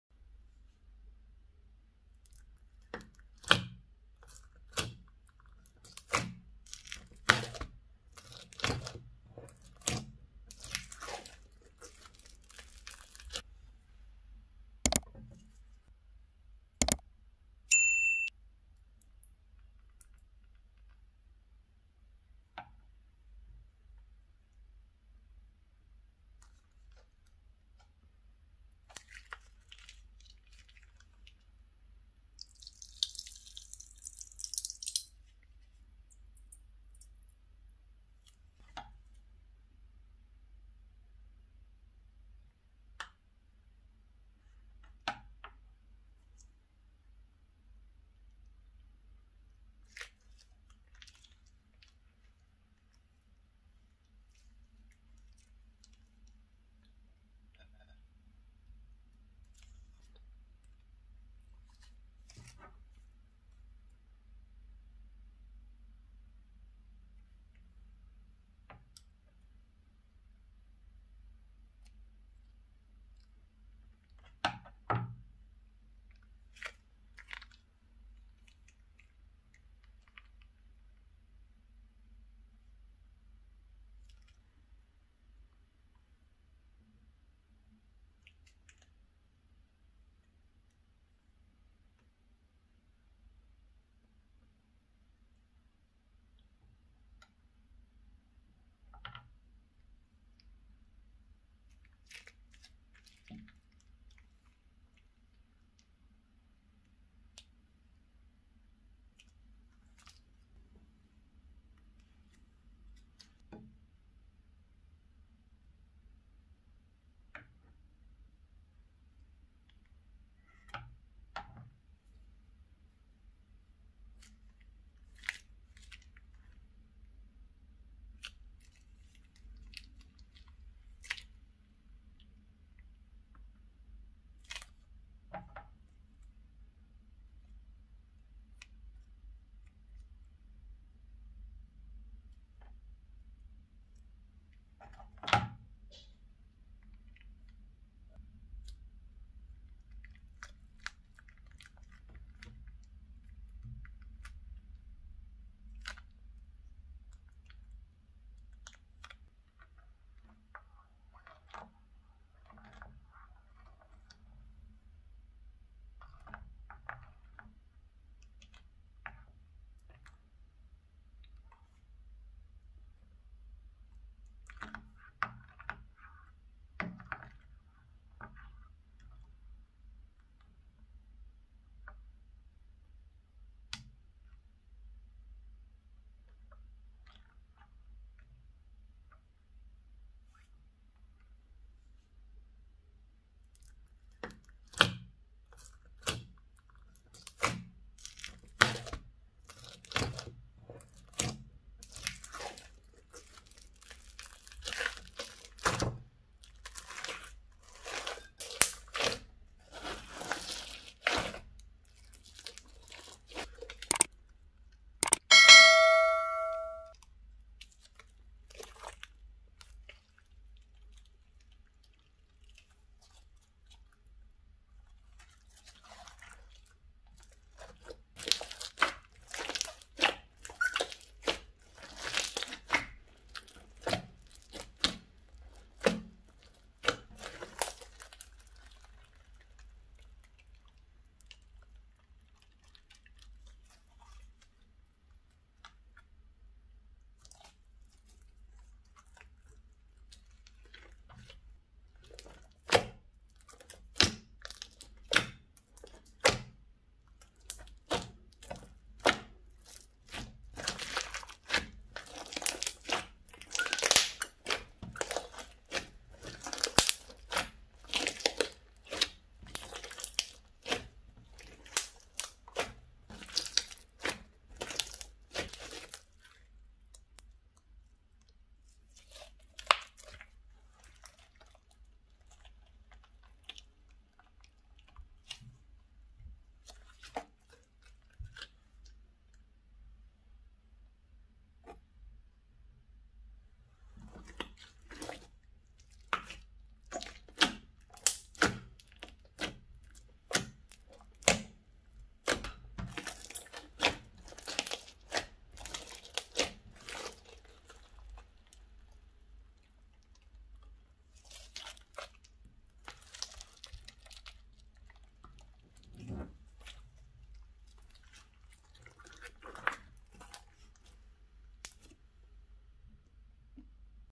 Making Glossy Pink Vs Blue Piping Bags Slime , ! ASMR !
Hi Slimeeeee , Goal is 1000 Subscriber please help me with it 💕
in this slime video you will see Mixing and making wonderful slime making with Blue VS Pink   piping bags to produce an amazing slime Swirl
if you want to see more slime video watch this links :

★ASMR★Most Satisfying Slime Video!